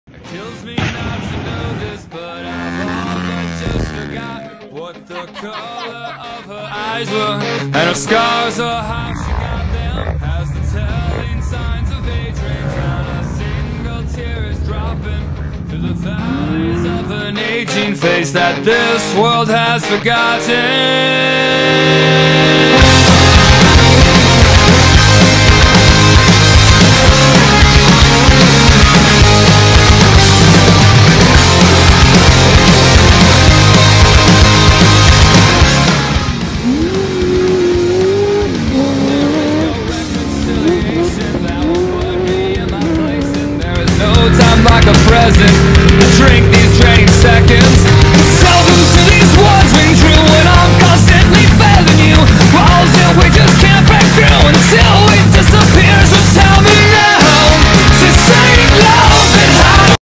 Glamis Thanksgiving 2020
Glamis Thanksgiving 2020 with sand cars unlimited Tatum Motorsports and Racer engineering sand cars.  Sand drags swing set sunset hill in high horsepower off-road cars playing in the best sand box on the west coast.